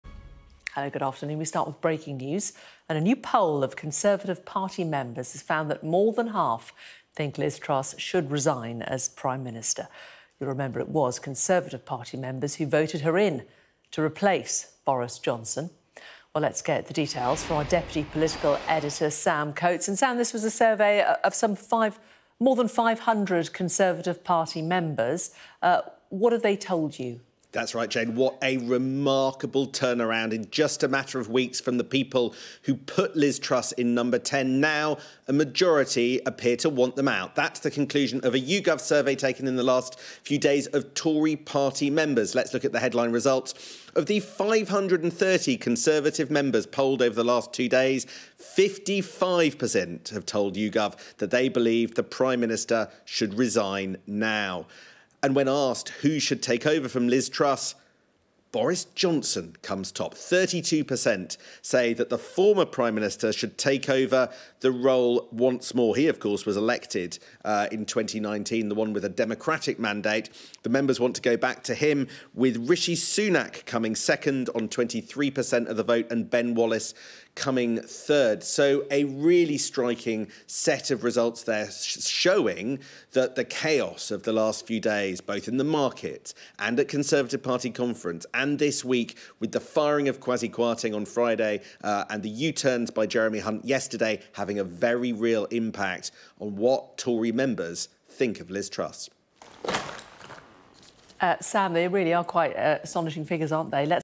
Tory Party members want Truss out - and Boris in
A poll of Conservative party members shows 55% want Liz Truss to quit now - and Boris Johnson to replace her.